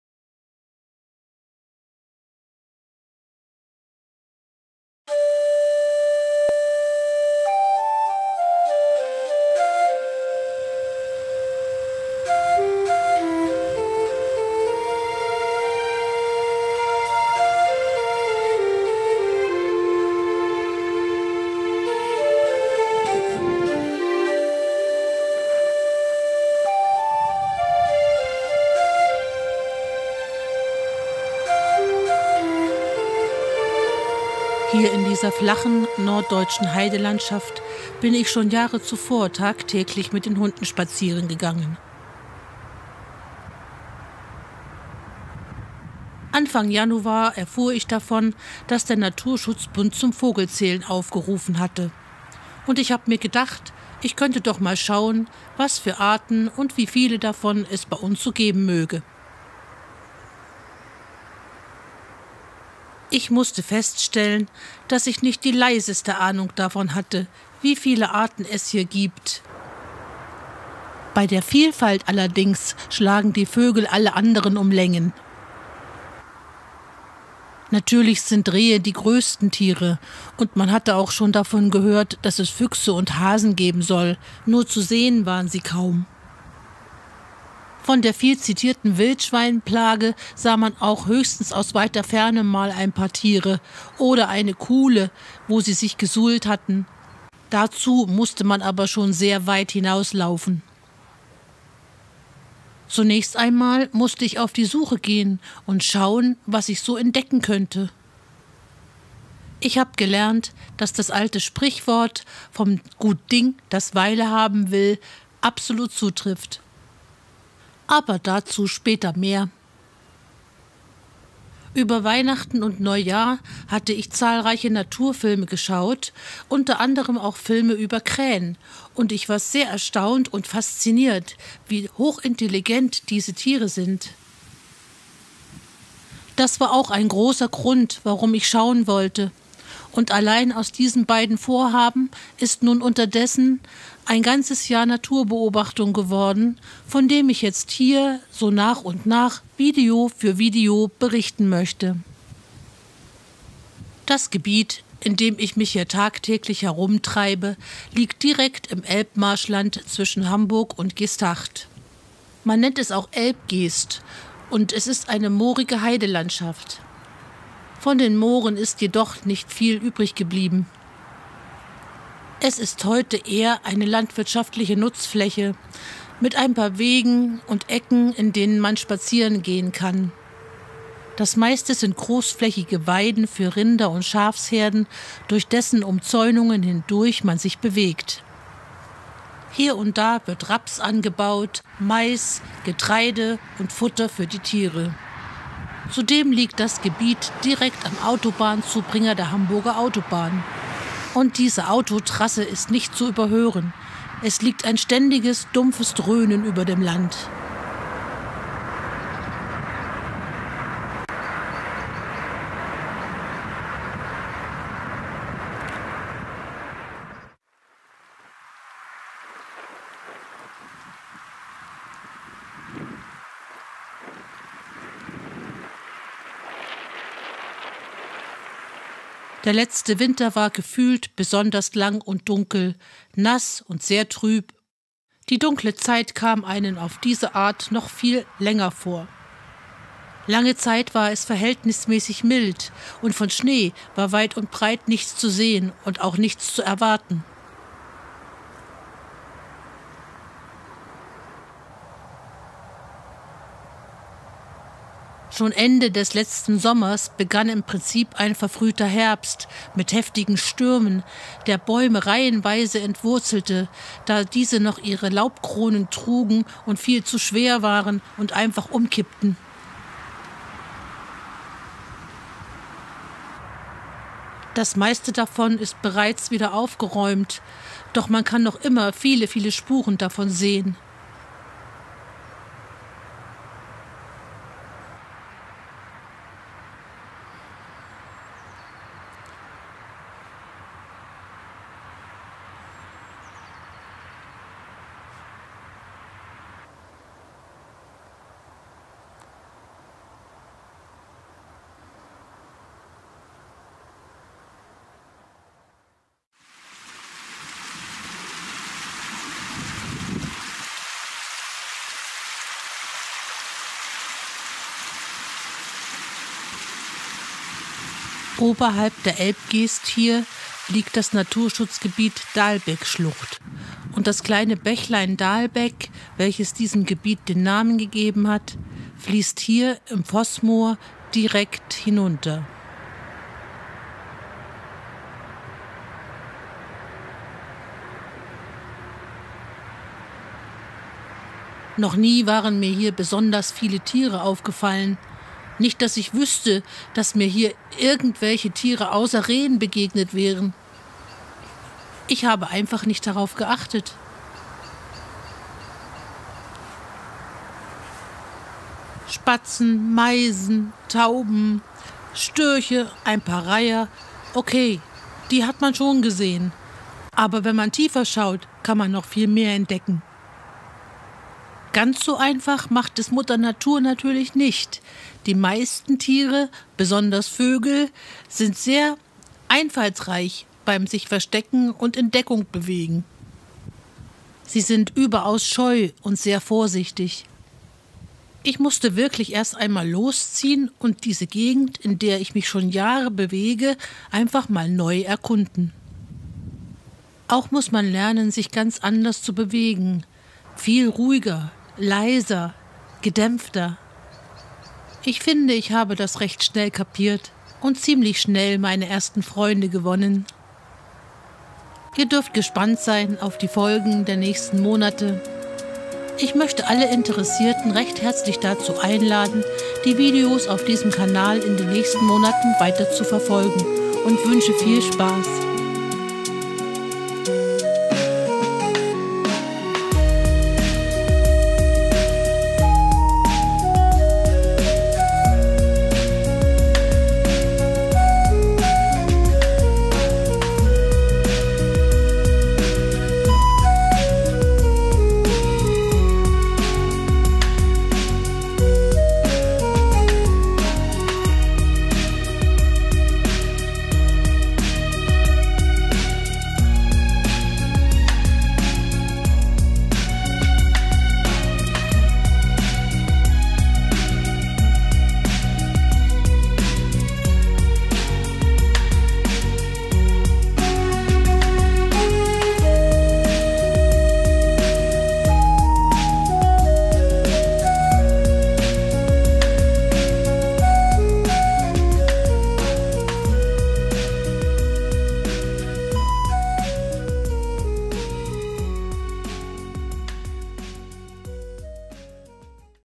Landschaftserkundung - Vossmoor Tagebuch Januar 2018
Das ist Teil 01 aus der Rubrik "Vossmoor - Tagebuch" hier auf meinem Kanal "Vossmoor Natur- Vlog" auf dem Du Videobeiträge über meine Naturbeobachtungen in der Moorlandschaft der Elbgeest südöstlich von Hamburg findest. Ich fasse meine Beobachtungen der Tierwelt, der Umwelt, der klimatischen Auswirkungen hier in Monatsabschnitten zusammen.
Klimawandel und Umweltverschmutzung sind derzeit das prägende Thema unserer Gesellschaft. Wie verändert z.B. unsere Landwirtschaft und überhaupt unsere Lebensweise direkt das Leben in unserer näheren Umgebung? Seit Januar 2018 habe ich mich täglich auf Erkundung begeben und durch das Einrichten einer Vogelfutterstelle unzählige kleine gefiederte Freunde gewonnen. Mit meiner Kamera habe ich versucht, das Leben in diesem kleinen, speziellen Gebiet zu dokumentieren.